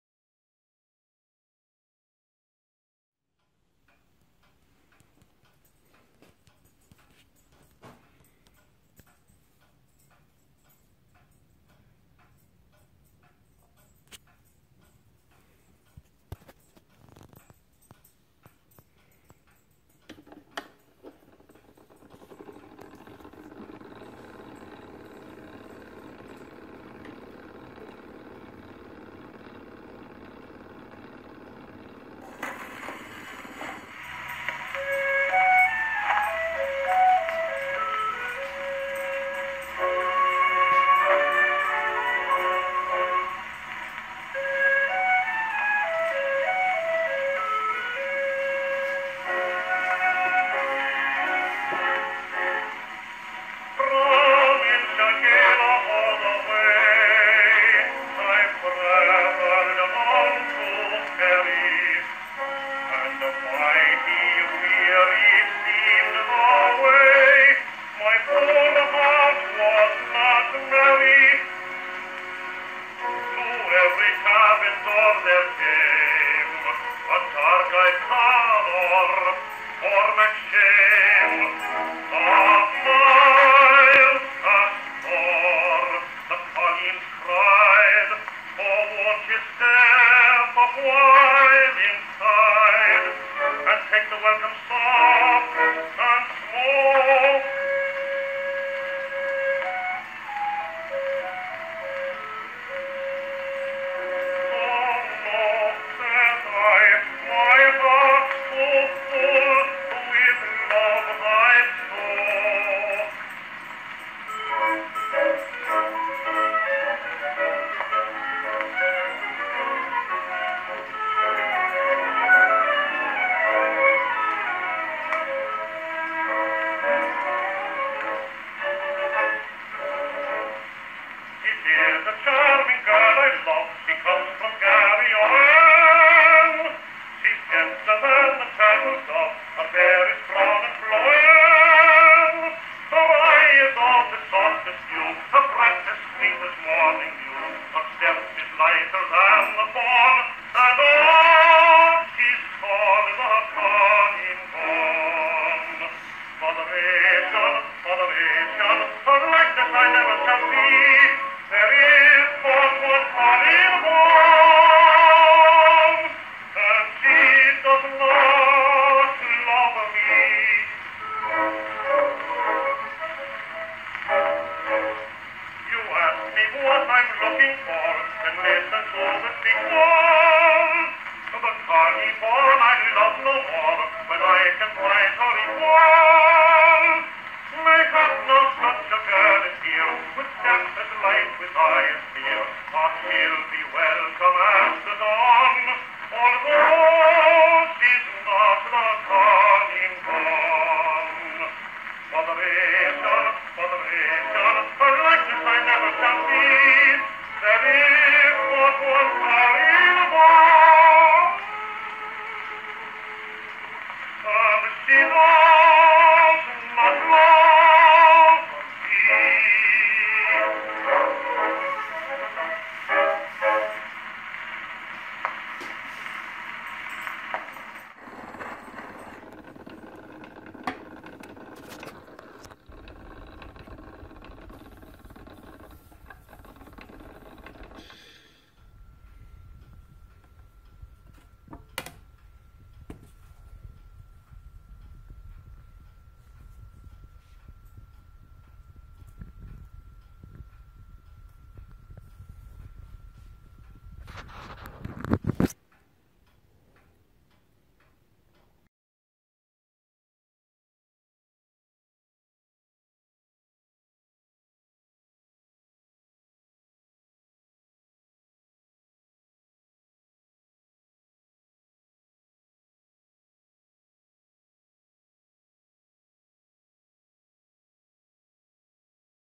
Victor I SN 9526
This is an attractive c.1903 Victor I phonograph with a very low serial number - 9526.

This is the earliest version of the Victor I featuring an 8" turntable with a beveled edge and forward crank. All the components are original. It is equipped with an Exhibition reproducer which has been rebuilt with new gaskets and back flange. It looks great and has been tuned for proper performance.  The turntable felt is original and in fair condition. The motor has been fully serviced and adjusted for optimum performance. The motor runs very strong, playing 10" records with ease. The original horn has some dents but still presents well.

The case is in great shape, all joints are solid.